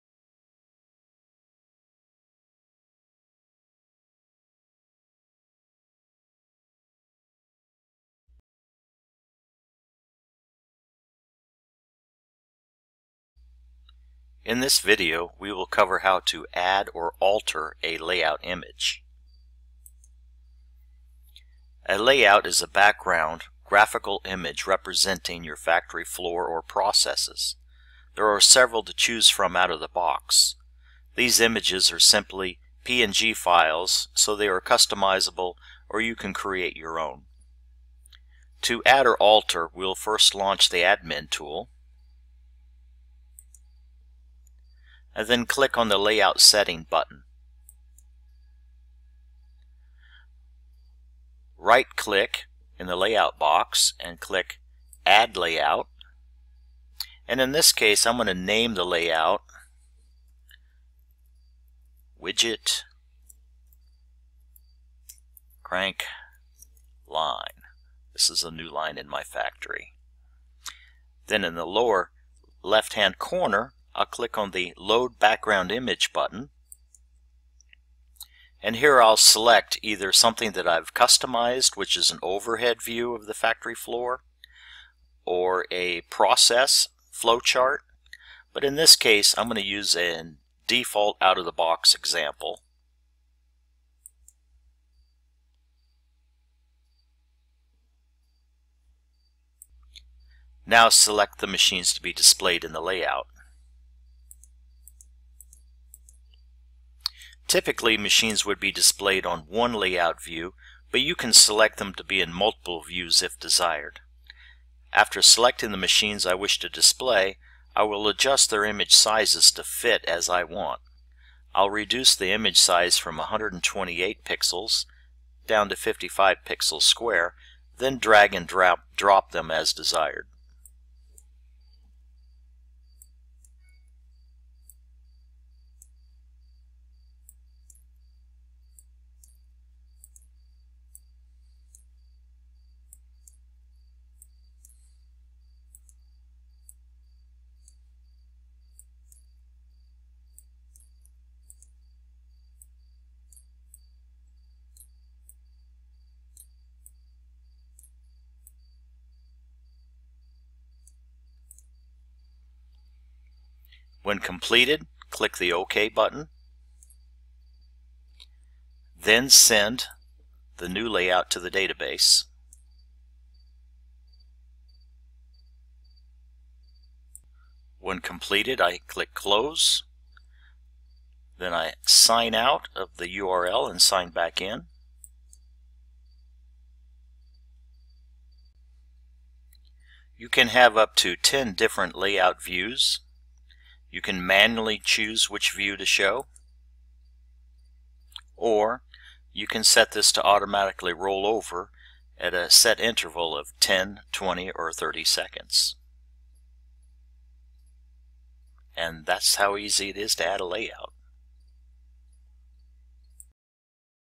MT-LINKi - How to add a new layout
Adding a shopfloor layout in FANUC MT-LINKi shopfloor connectivity platform.